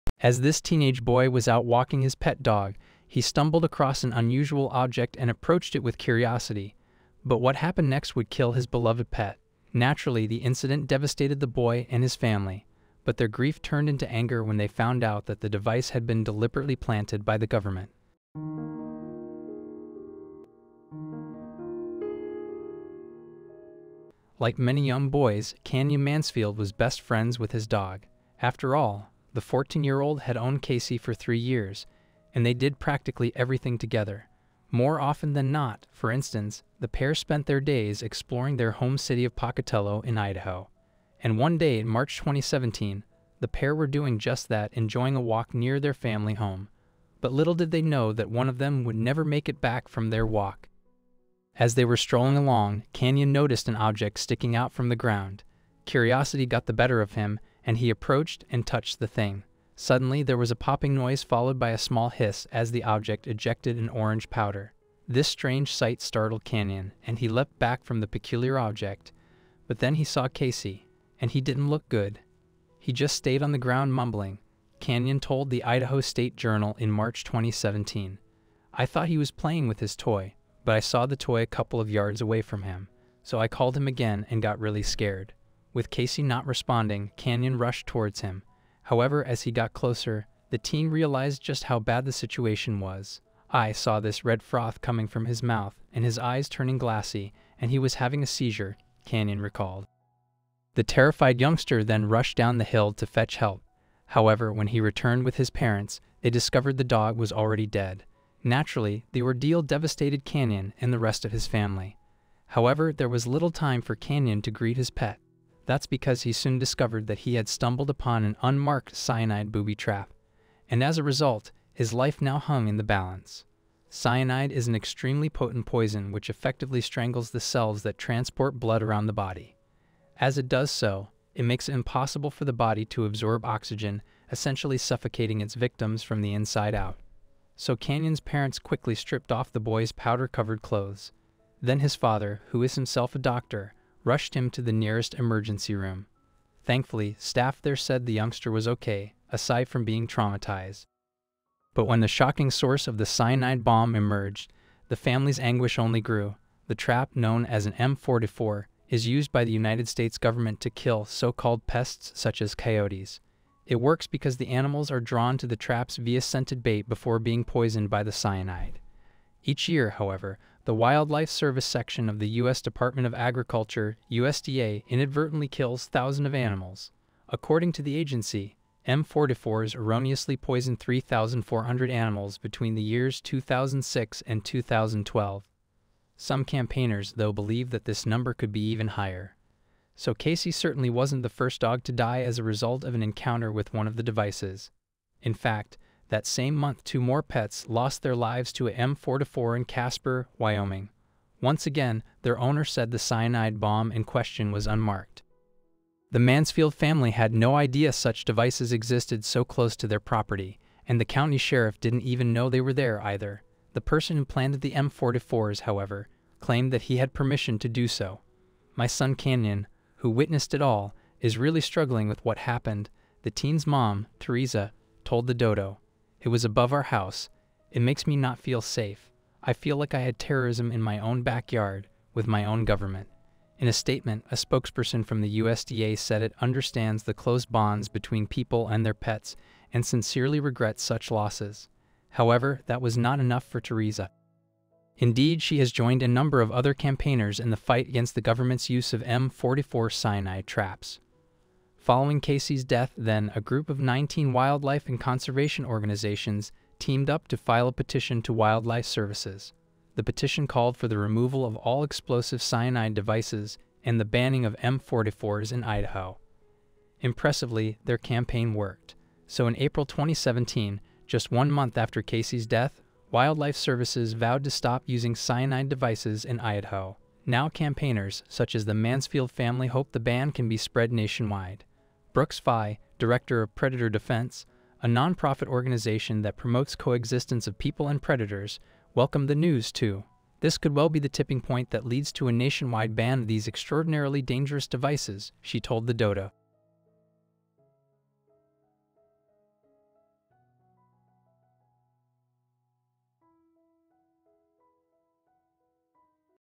This Strange Tool Ki*led A Family’s Dog and They Found Out The Government Put It There
This Strange Tool K*lled A Family’s Dog and They Found Out The Government Put It There